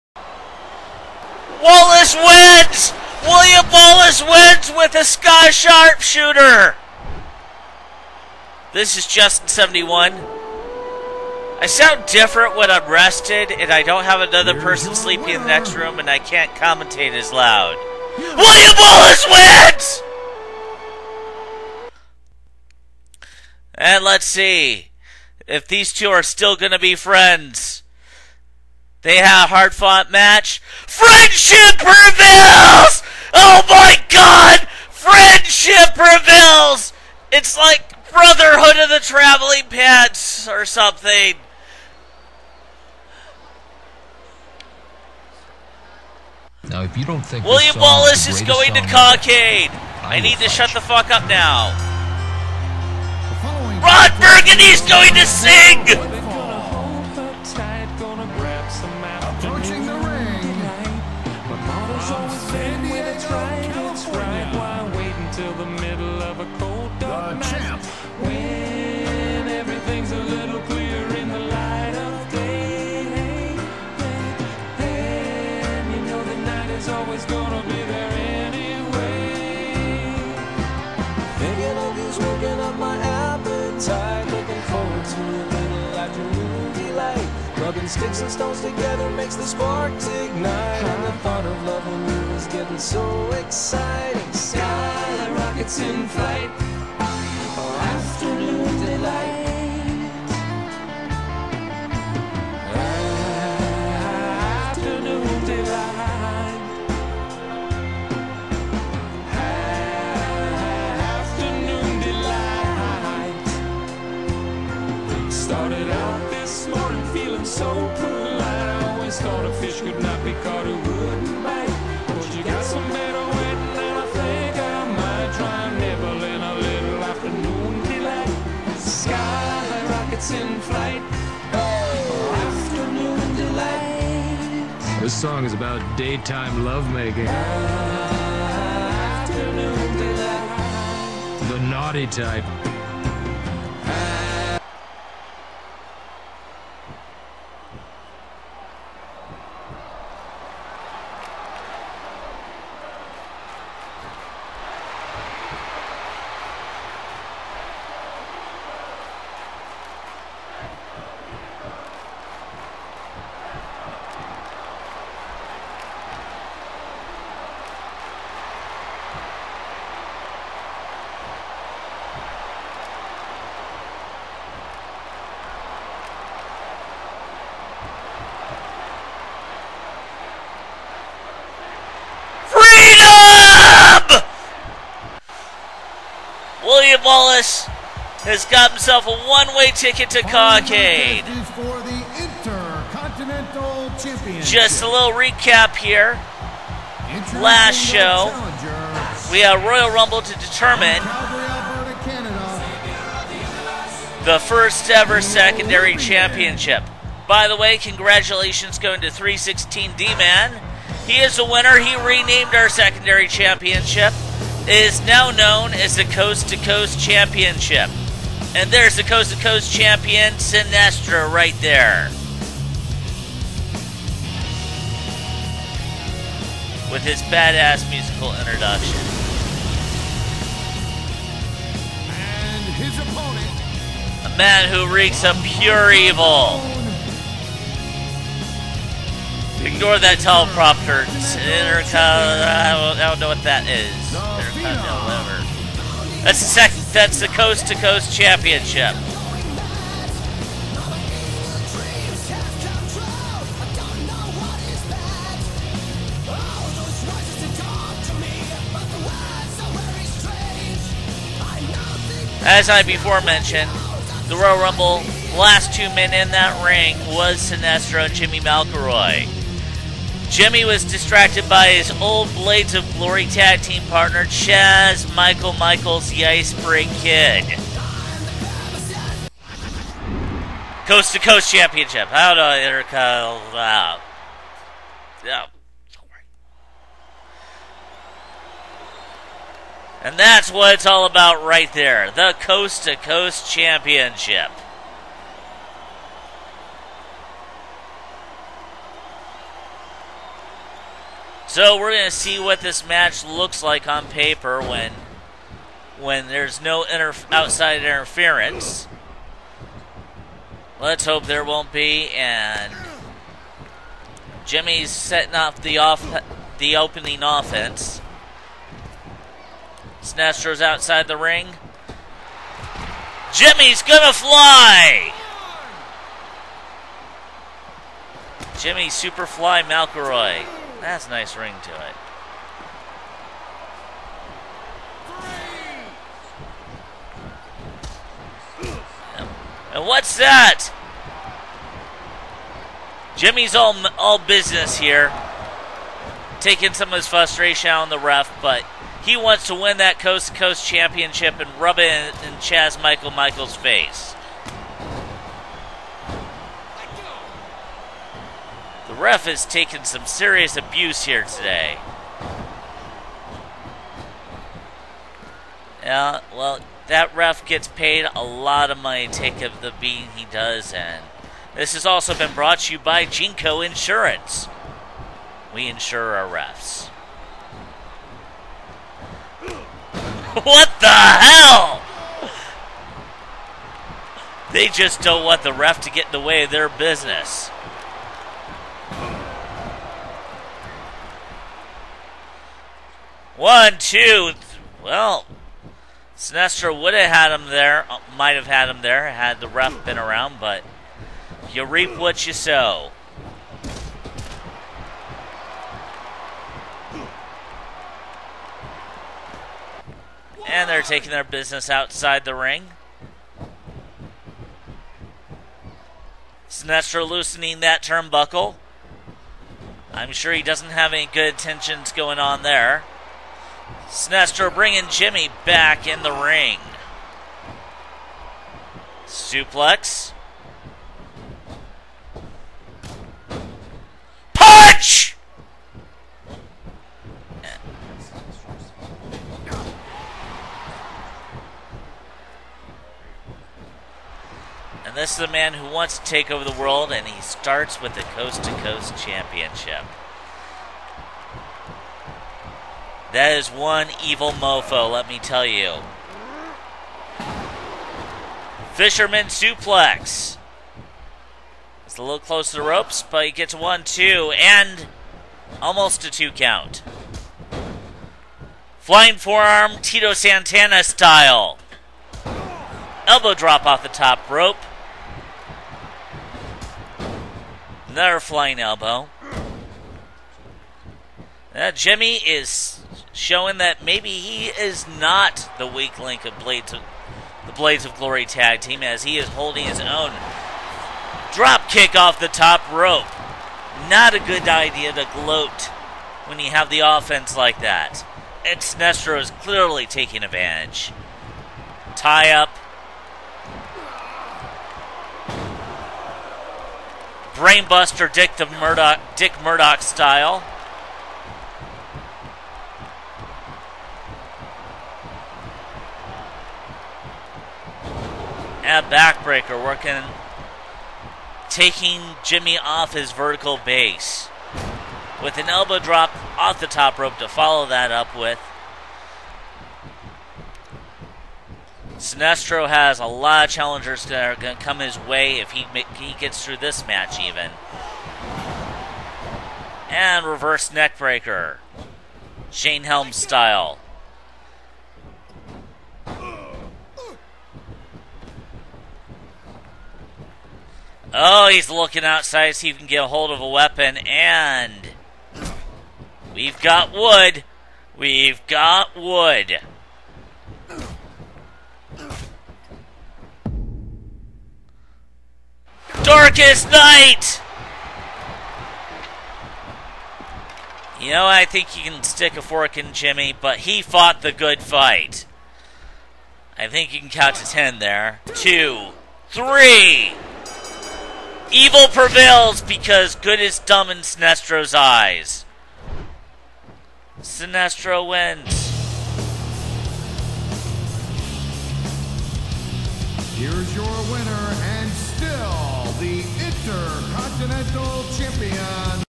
Ig-Nitro Episode 4 pt 4
NoDQ Street Fight Cade FTW Skywalker vs Johnny Lawrance

NCA World title Semi-finals Tourney
Superman Prime vs Graverobber
Guy Gardner vs William Wallace

Coast 2 Coast title Sinestro(c) vs Jimmy MacElroy
Non-Title Jonas Brothers vs Terance & Phillip